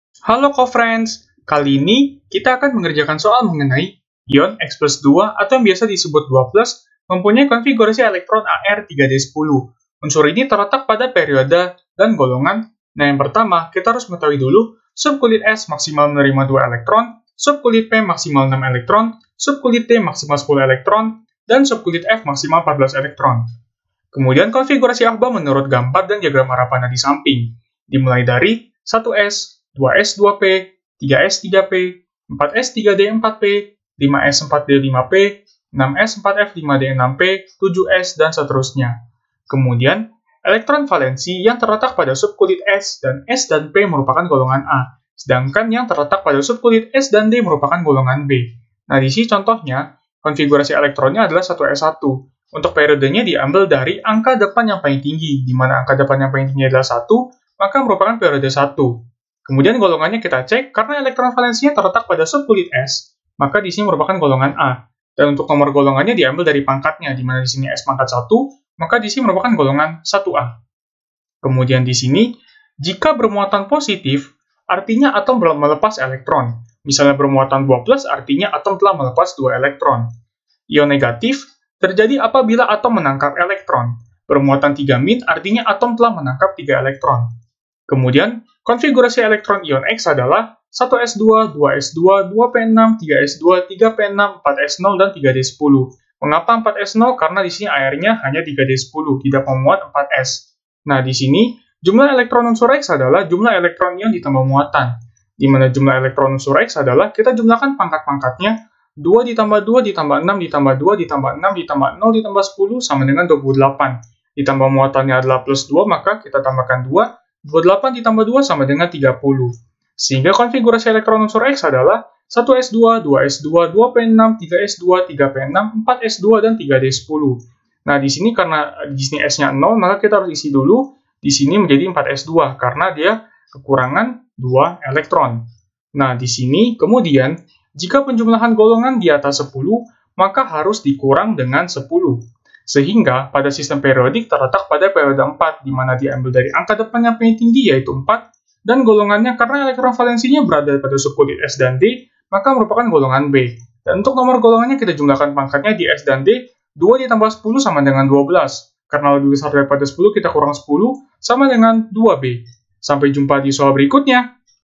mempunyai konfigurasi elektron [Ar] pada perioda dan golongan. Ion Xt2 3d10 unsur ini terletak
Topik atau Materi: Hubungan Konfigurasi Elektron dengan Letak Unsur dalam Tabel Periodik - Struktur Atom dan Tabel Periodik - Kimia Anorganik - Kimia Kelas 10 SMA -

Sumber Soal: Kimia 123dok , N/A

00:00 : Bedah Soal
01:00 : Konsep, rumus dan pengertian Hubungan Konfigurasi Elektron dengan Letak Unsur dalam Tabel Periodik
01:18 : Langkah penyelesaian soal
01:44 : Jawaban akhir
02:14 : Penutup